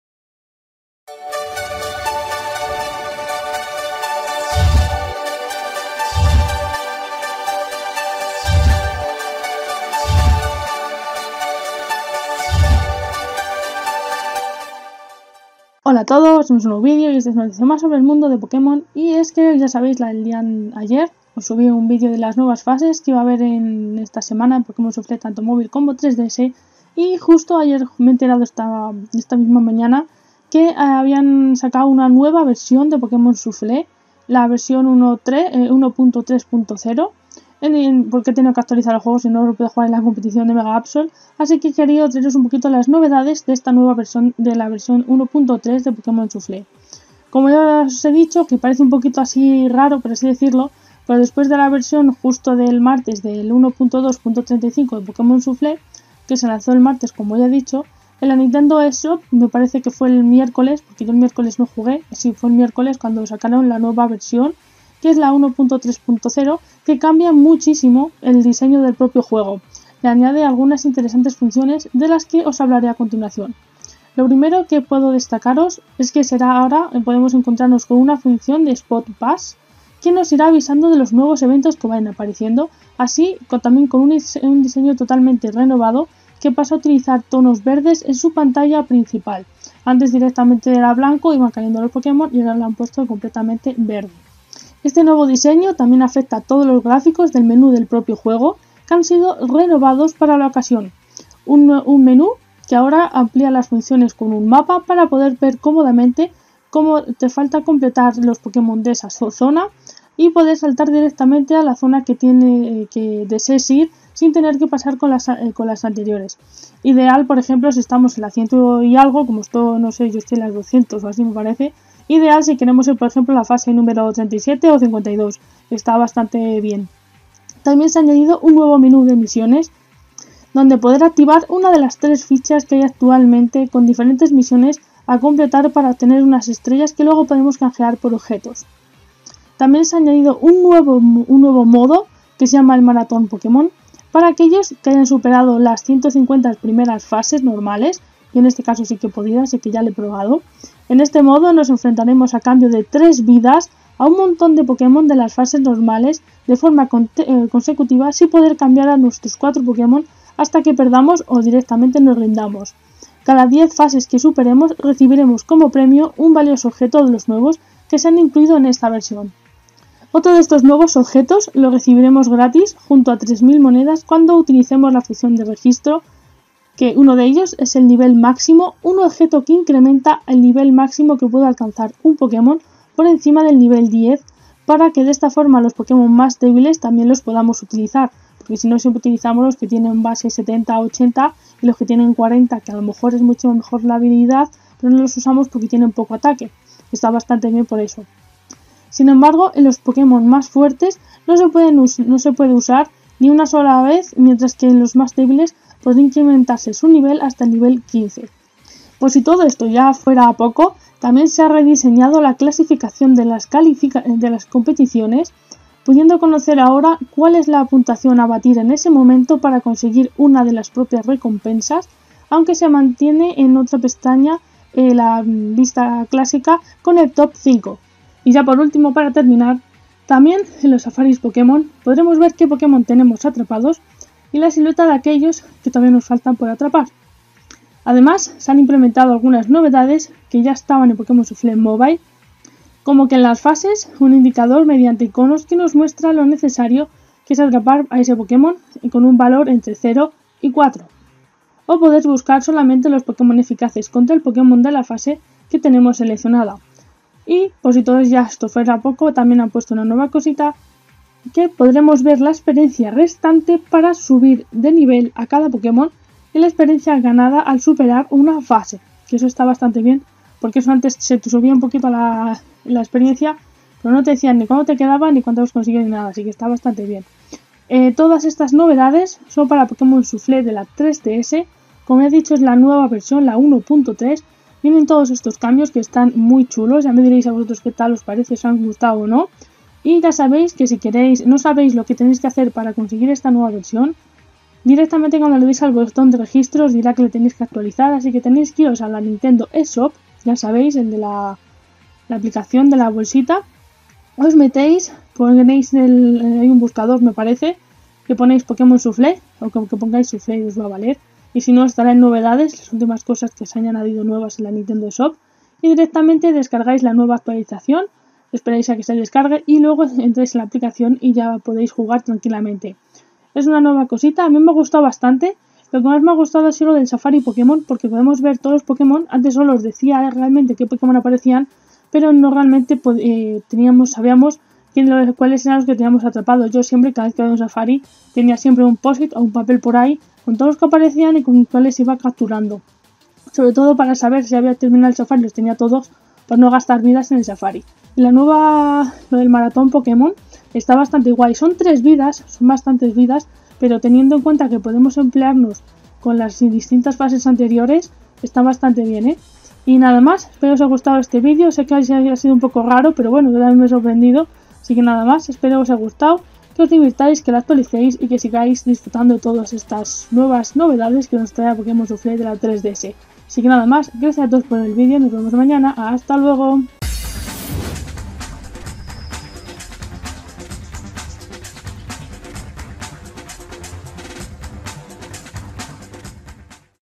Pokémon Shuffle llega con muchas novedades en la versión 1.3
Os cuento sobre las novedades que se incluyen en esta nueva versión.